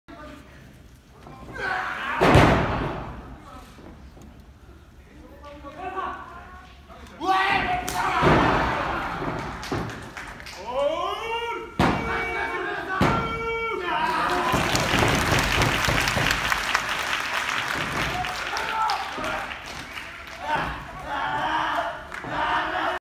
竹下幸之介がジャーマンスープレックスからザーヒーを決めるも3カウントは許さず絞め技へ！ DDT「D王 GRAND PRIX 2021 Ⅱ the FINAL」
メインイベント
D王 GRAND PRIX 2021 Ⅱ優勝決定戦
竹下幸之介 VS 上野勇希
DDT「D王 GRAND PRIX 2021 Ⅱ the FINAL」 2021年12月05日@後楽園ホール